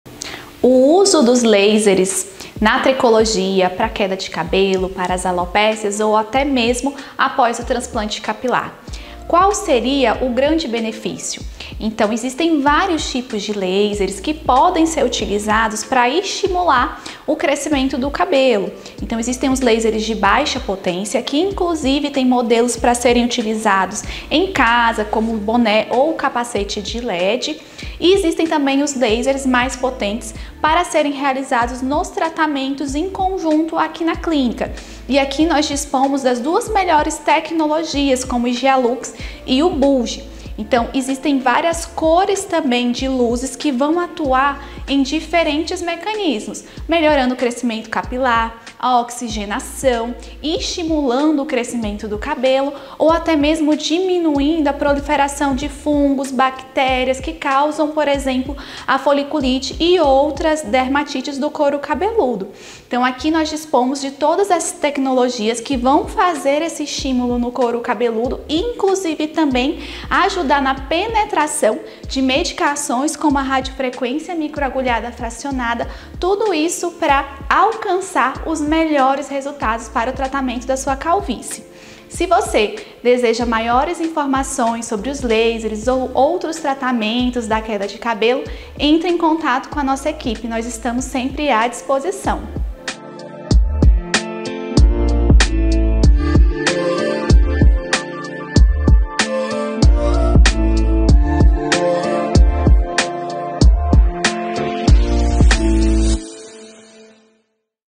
O uso de Lasers no Tratamento Capilar - Dra. Thalita Carlesso
👉 Nesse video você vai entender os benefícios do uso de laser de baixa e alta intensidade para estimular o crescimento de cabelo!

A clínica Dra. Thalita Carlesso dispõe das tecnologias inovadoras, Hygia Lux e Bunge, para tratar e auxiliar no seu tratamento capilar!